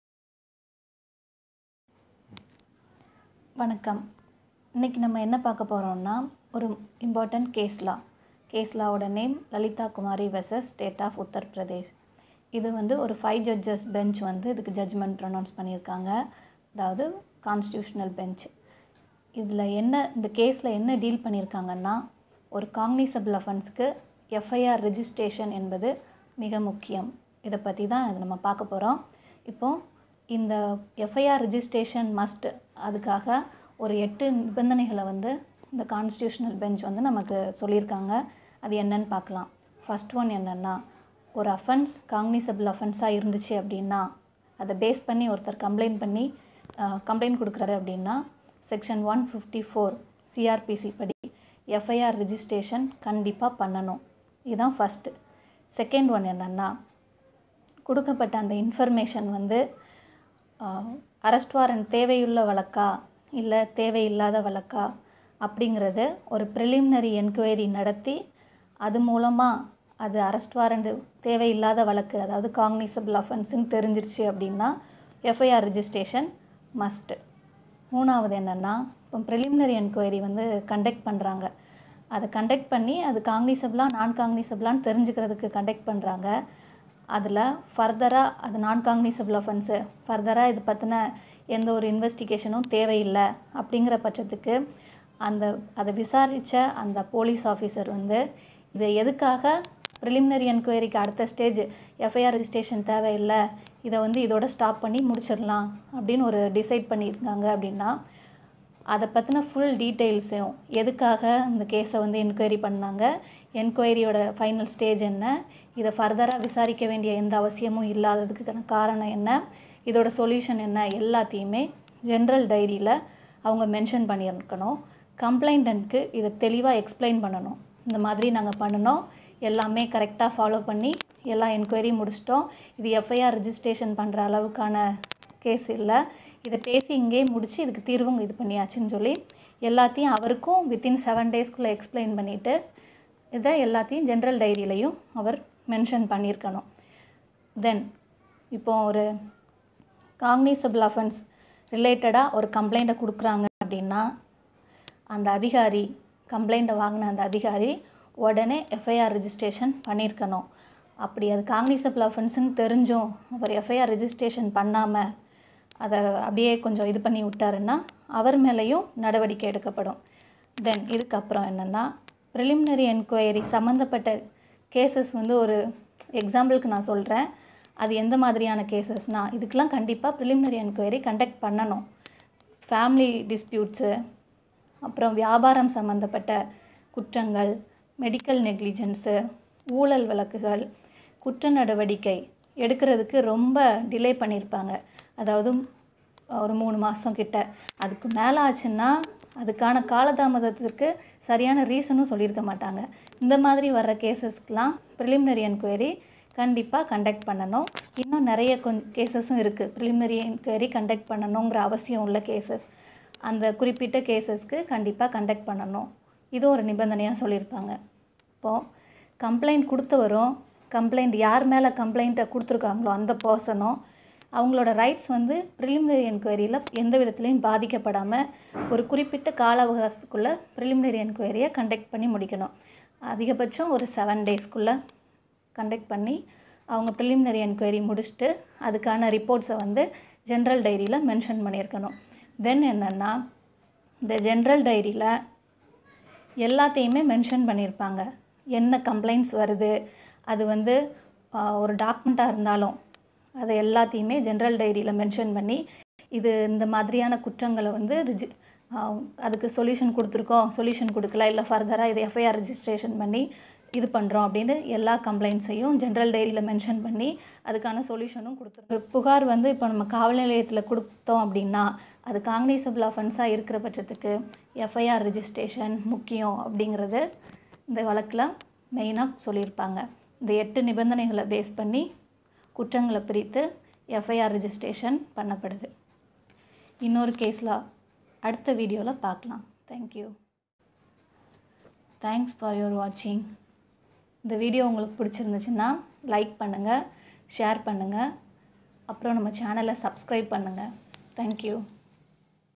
நீங்கள் கொடுக்கும் புகாருக்கு உடனடியாக FIR பதிவு செய்ய வேண்டுமா?!..
நீங்கள் கொடுக்கும் புகாரில் (Cognizable Offense) இருக்குமாயின் அந்தப் புகாரின் அடிப்படையில்
உடனடியாக FIR பதிவு செய்ய வேண்டும்- உச்சநீதிமன்ற தீர்ப்பு: